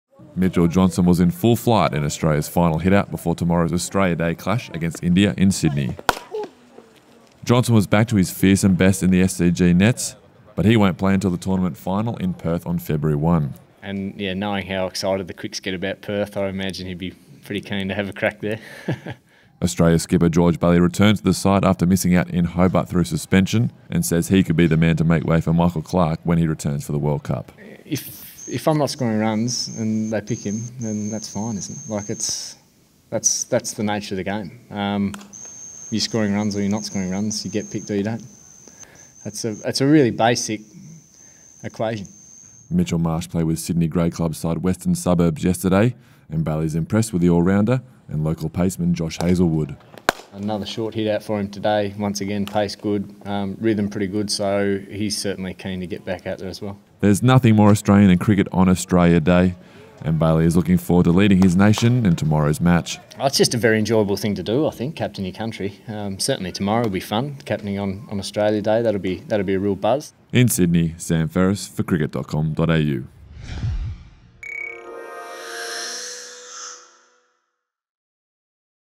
Bailey and co all set for Australia Day
Australia enjoyed a solid training run ahead of their SCG clash with India tomorrow as Mitchell Johnson, Mitchell Marsh and Josh Hazlewood all had solid hit-outs